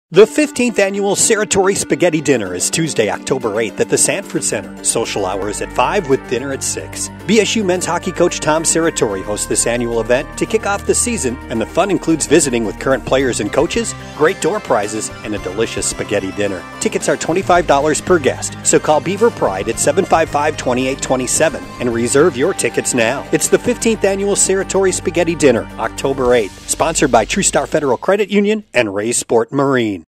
2019 Serratore Spaghetti Feed
TV ad for the 2019 Serratore Spaghetti Feed for Bemidji Sate University Men's Hockey